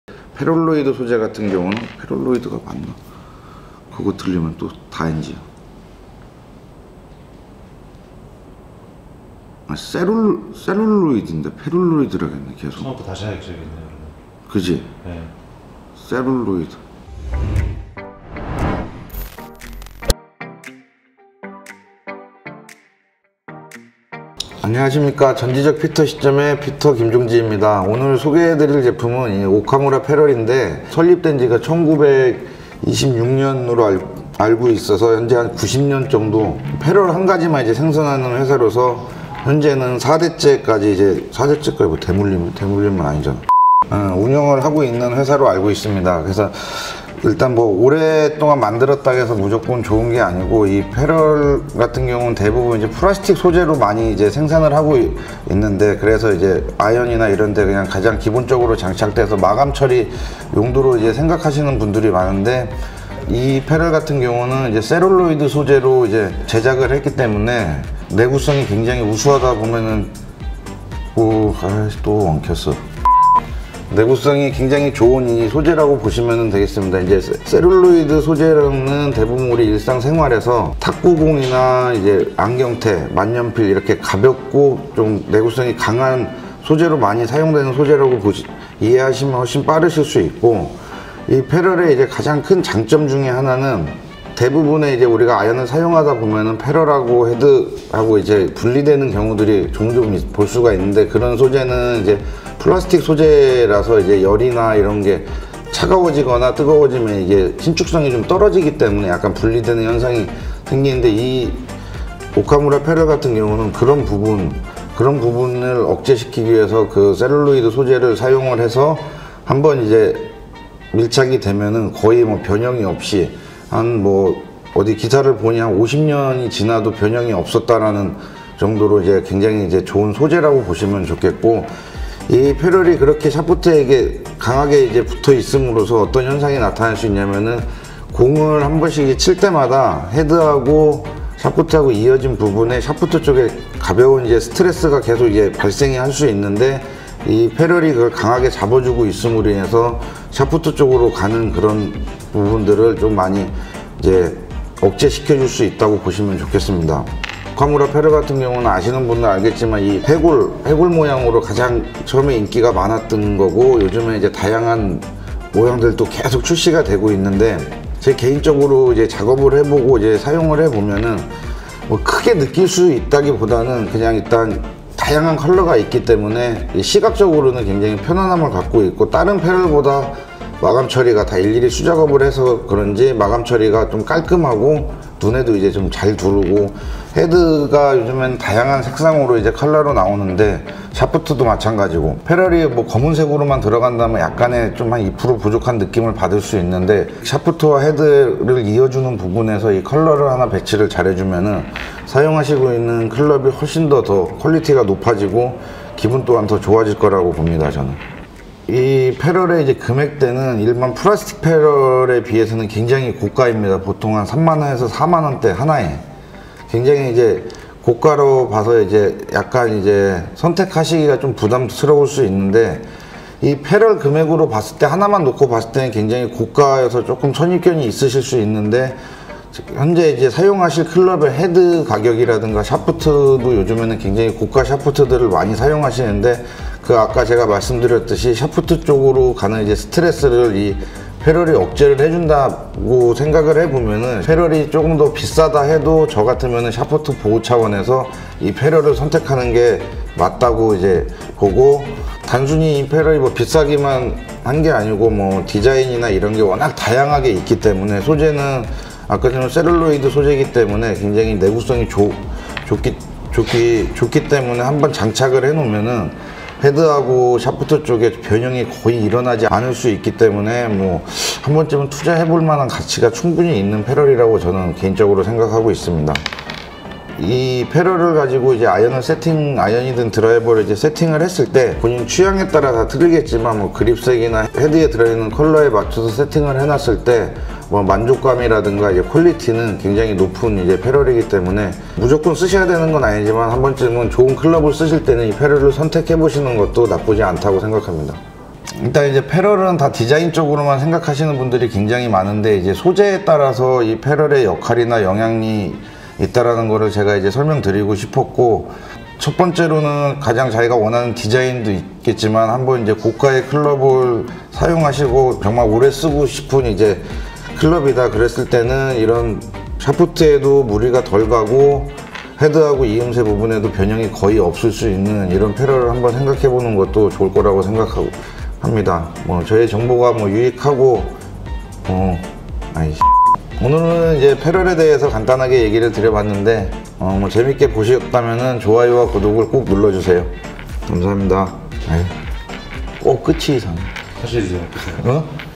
[리뷰] 아직도 페룰 아무거나 끼세요? 셀룰로이드 페룰의 튼튼함! 오카무라 페룰 (Okamura Ferrule review)
전지적 피터 시점

이번 에피소드는

아직도 페룰은 대충 끼우는 당신!
셀룰로이드 페룰의 내구성과
디자인의 끝판왕
오카무라를 소개합니다!